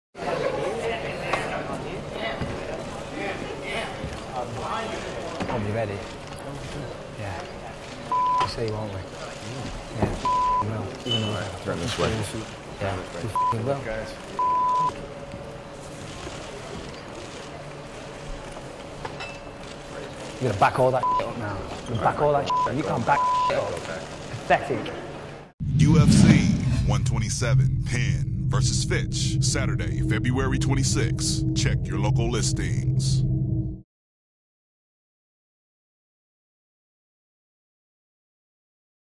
UFC 127 Tempers Flare
With the fight only days a way, Michael Bisping and Jorge Rivera can't wait to step into the Octagon.  Watch UFC 127: Penn vs Fitch Live on Pay-Per-View at 7pm PT/ 10pm ET this Saturday, February 26th.